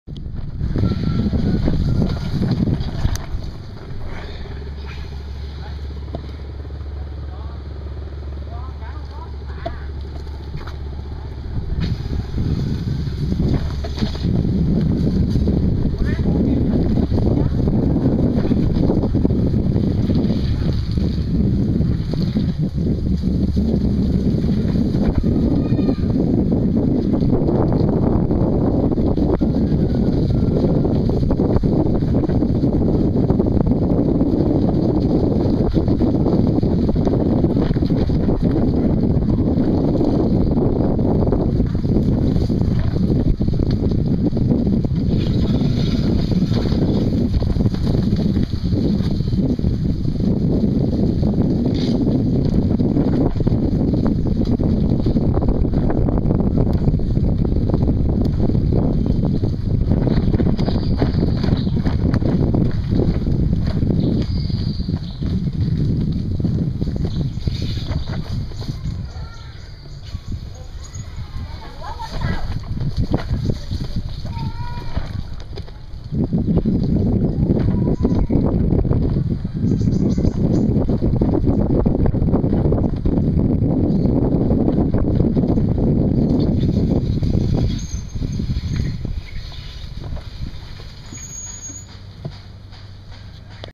Fietsen in Vietnamese natuur
Zo zie je eens wat hier allemaal groeit